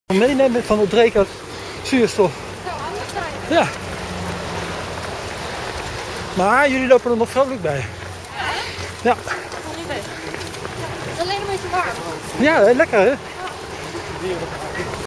Zuurstofdames tijdens CPC 2012
Dräger tijdens CPC leveren, die geen zuurstof, zo vroeg ik deze vrolijke loopsters.